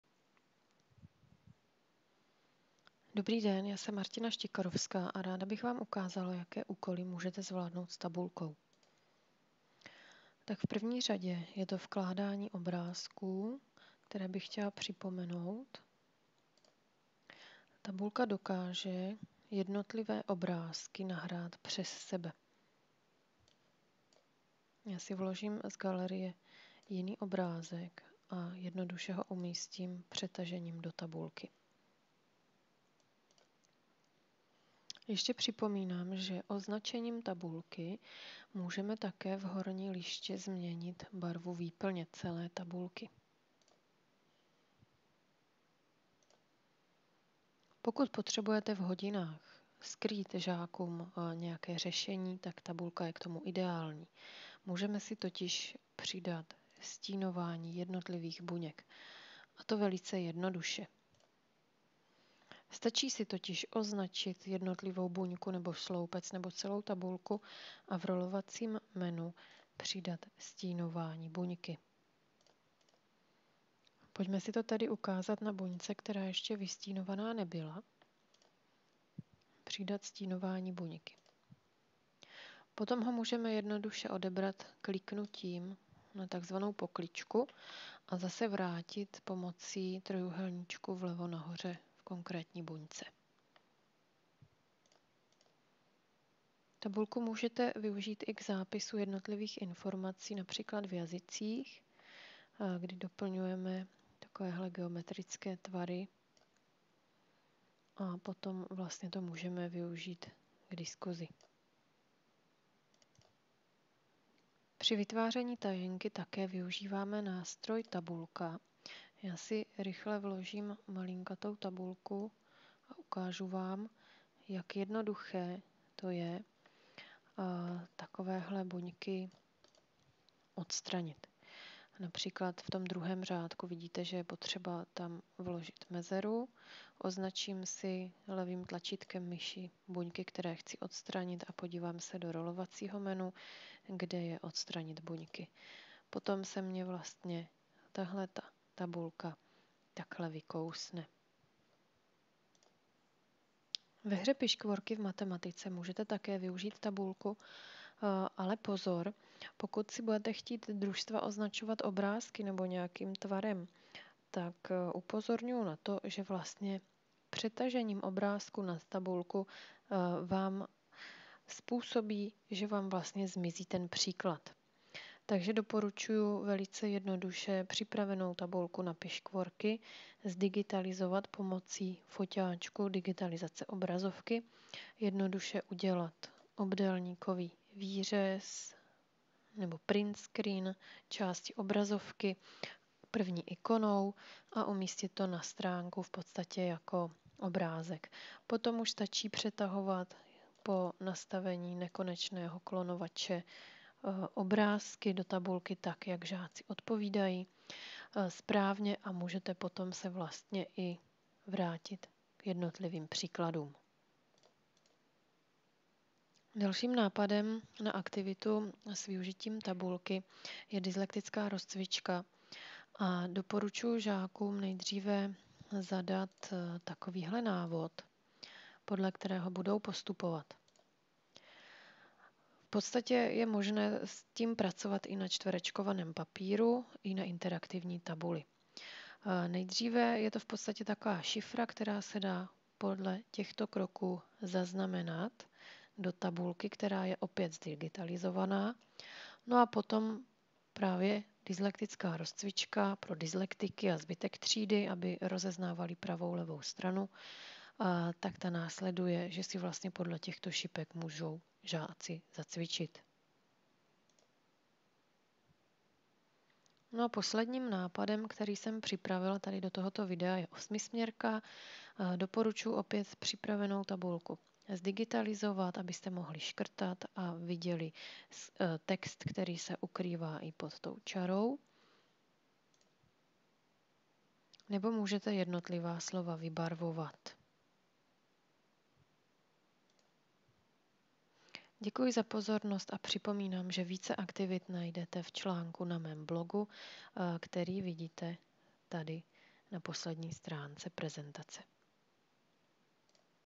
SMART Notebook tabulka
Netradiční využití základních nástrojů SMART Notebooku - tabulka.